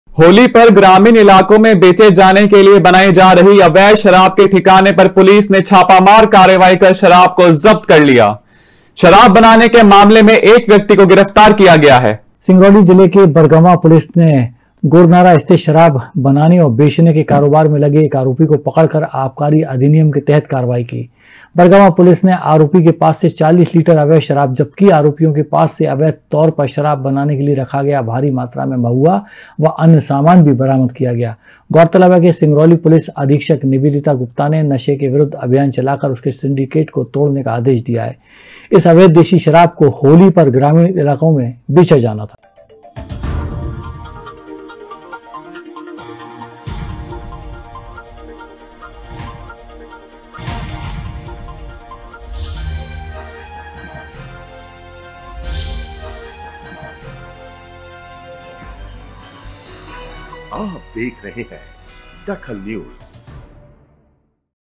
DAKHAL NEWS होली के लिए तैयार की गई थी देसी शराब
होली के लिए तैयार की गई थी देसी शराब
ग्रामीण अंचलों में बिक्री के लिए बनी थी शराब